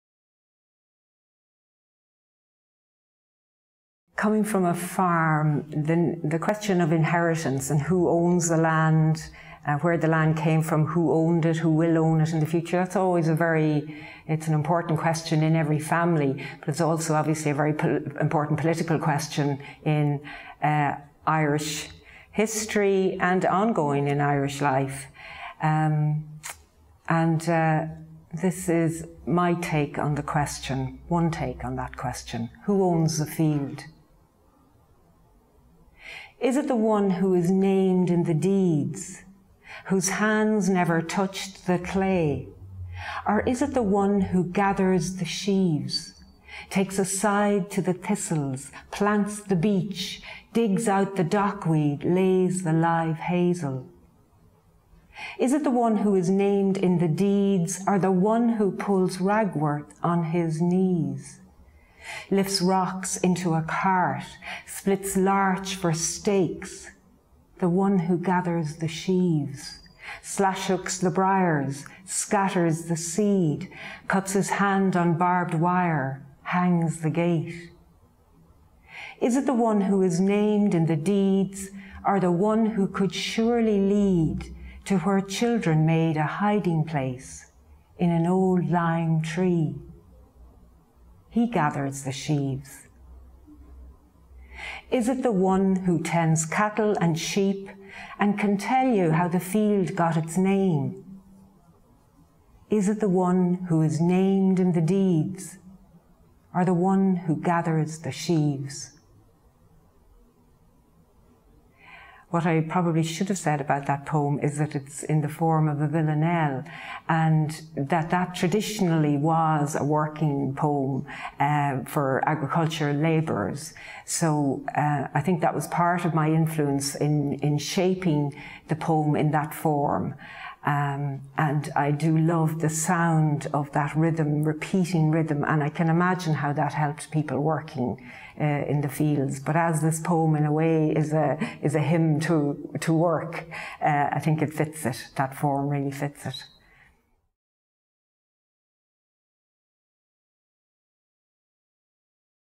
"Who owns the field?", read by Jane Clarke
Jane Clarke reads her poem "Who owns the field?" in UCD's James Joyce Library. Part of the Irish Poetry Reading Archive.